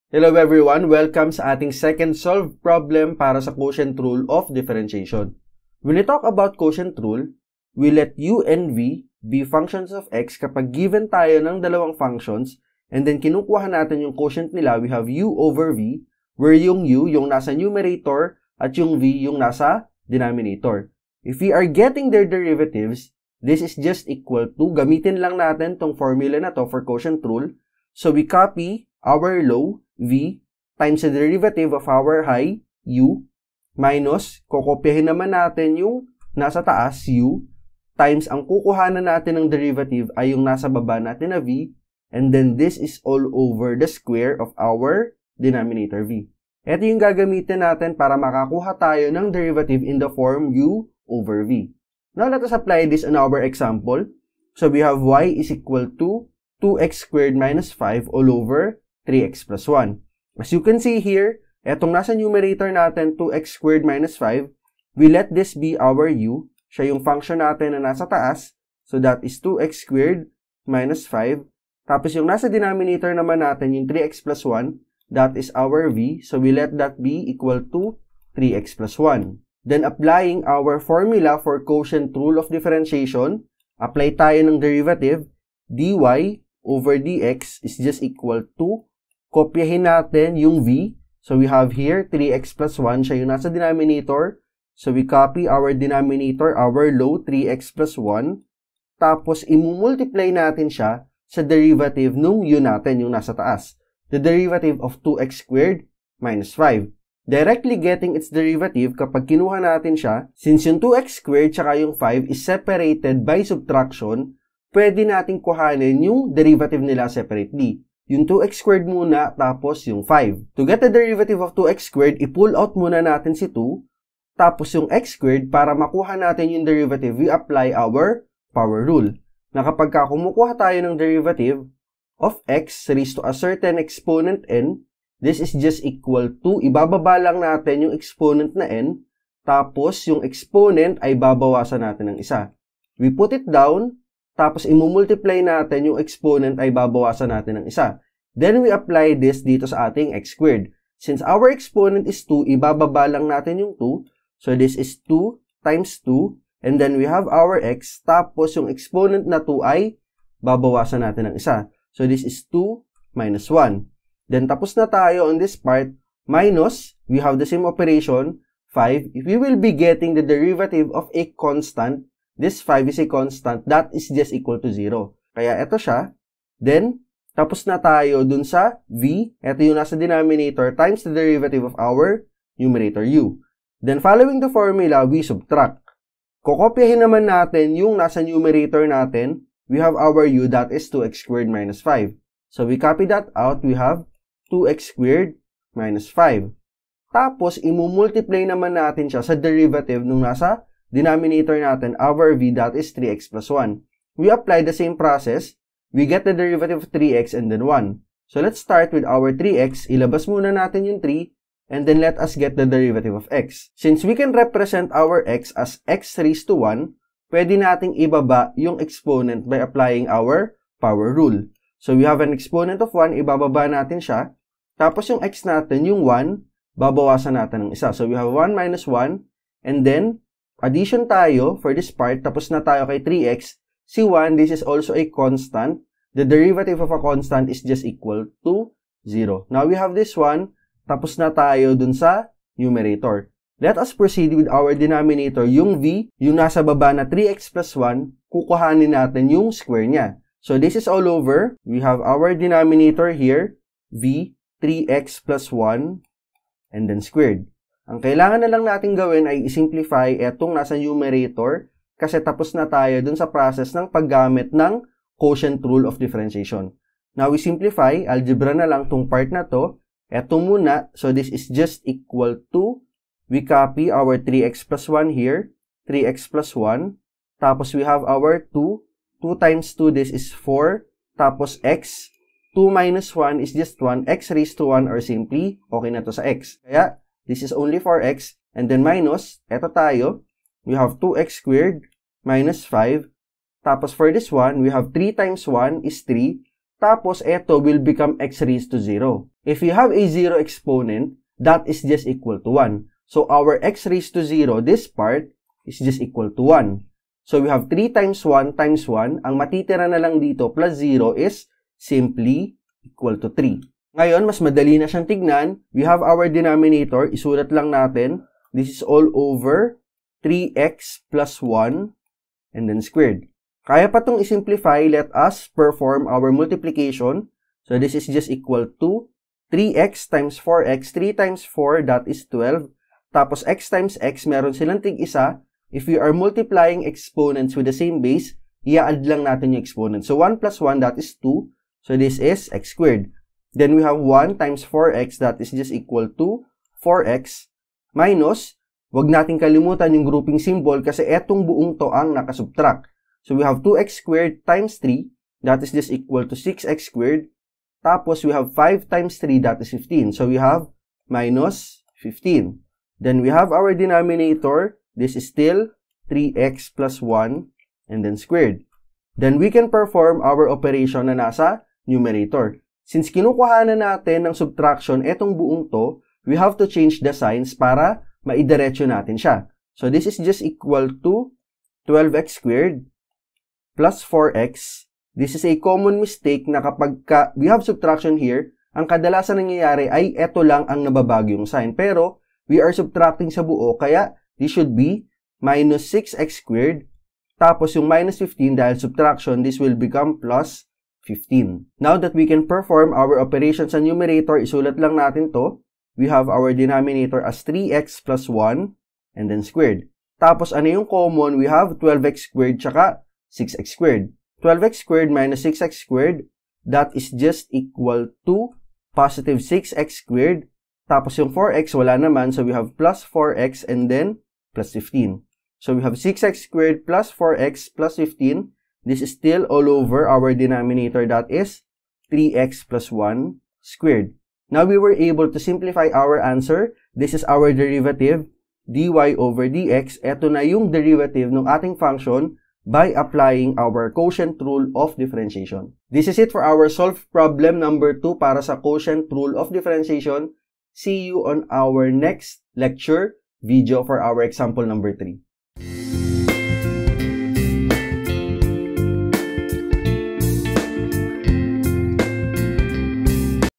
Solved # 2: Quotient Rule of Differentiation - Basic/Differential Calculus
A video discussing the use of the quotient rule of differentiation to solve the derivative of functions. This lesson is under Basic Calculus (SHS) and Differential Calculus (College) subject. Discussed in mixed Filipino/Tagalog and English. Designed for Flipped Classroom.